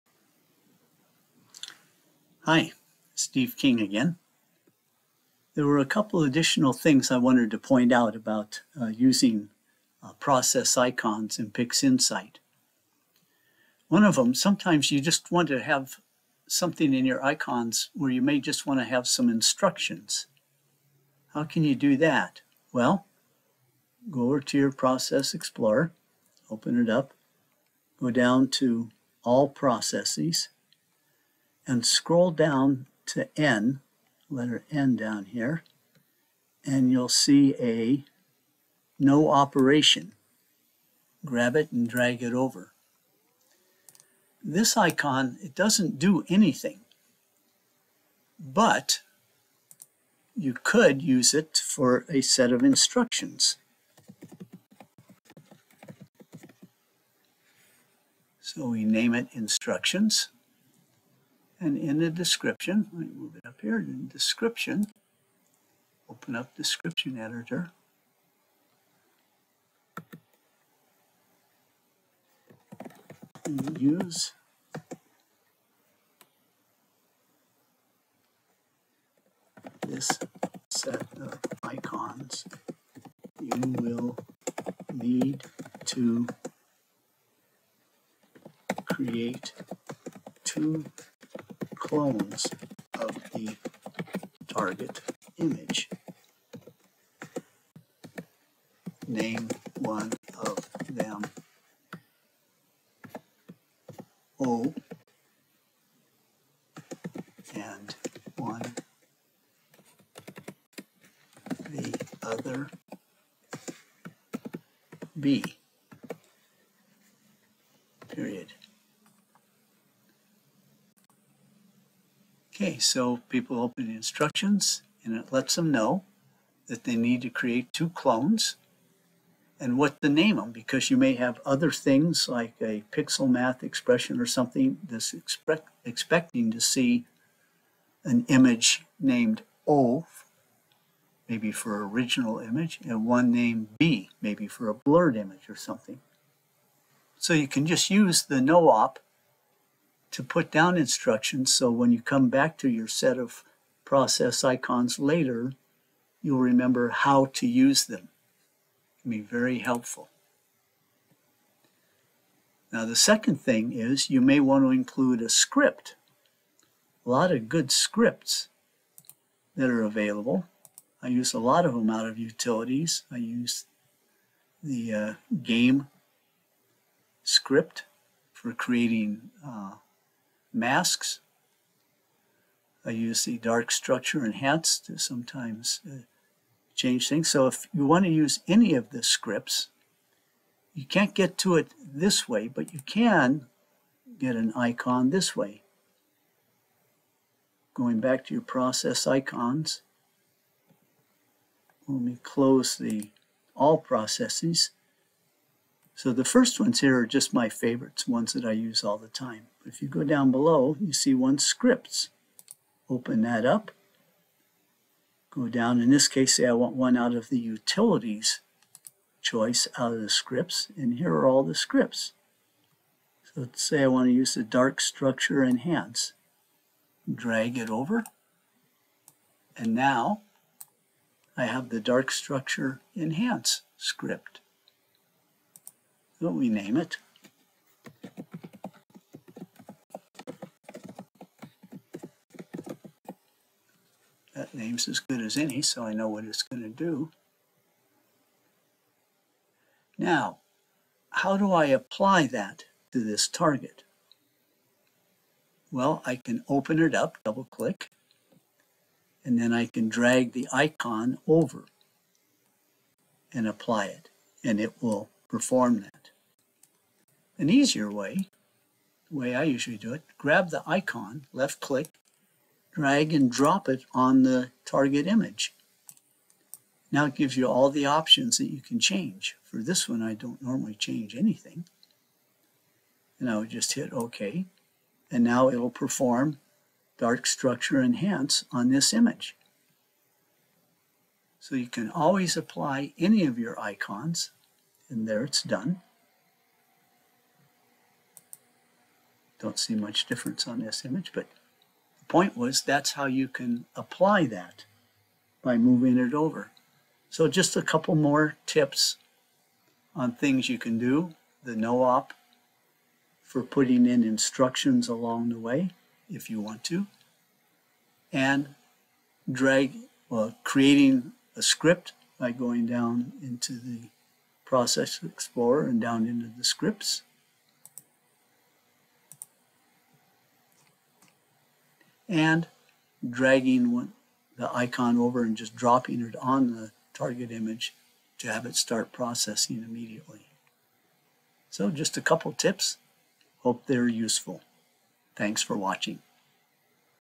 Creating, Editing, Saving and Loading PixInsight Process Icons: Part 2
A few additional tips about using PixInsight's process icons.